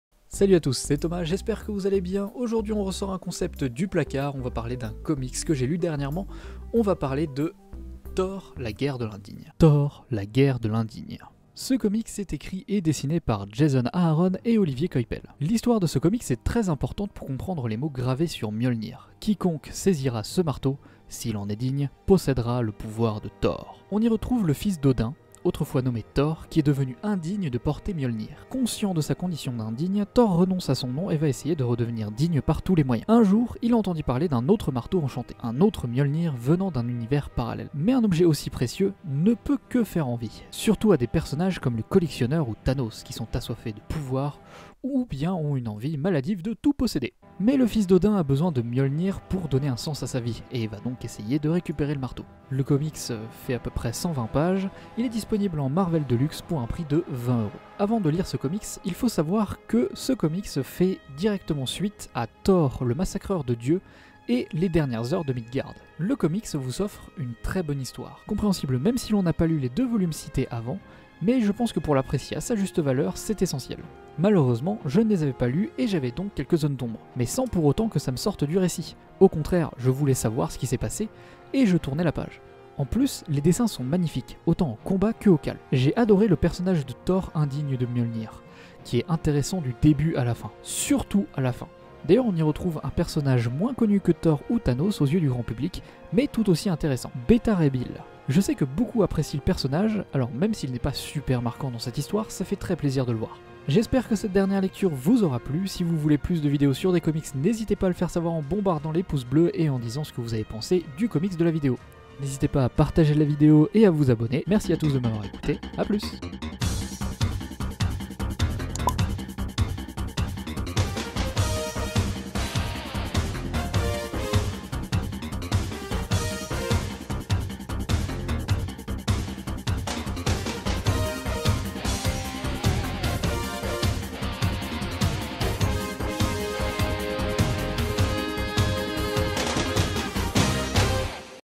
THOR, la GUERRE de l'INDIGNE : Dernière Lecture Comics #3
Salut à tous c’est thomas, aujourd’hui on ressort un concept du placard pour parler d’un comics que je viens de finir, j’ai nommé Thor la guerre de l’indigne.